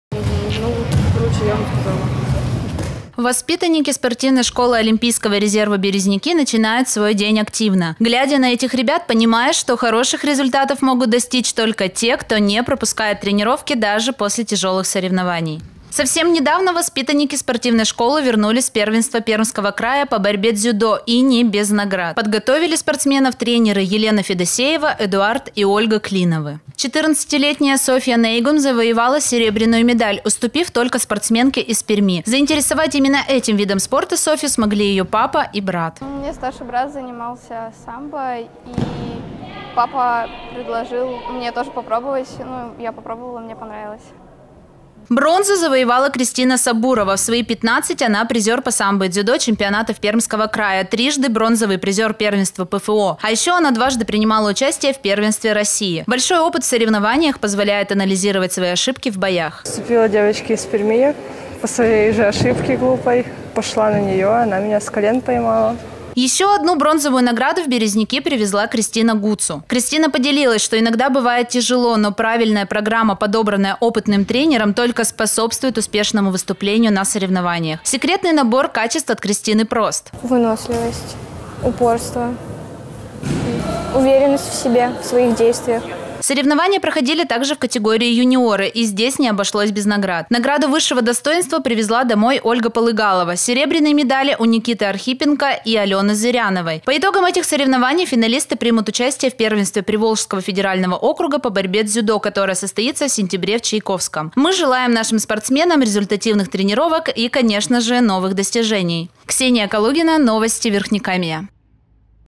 Дзюдо
И новые победы наших спортсменов! С 19 по 21 августа в Чайковском проходило первенство Пермского края по борьбе дзюдо среди юношей и девушек до 18 лет и юниоров до 21 года. С теми, кто привез заветные медали встретилась Ксения Калугина.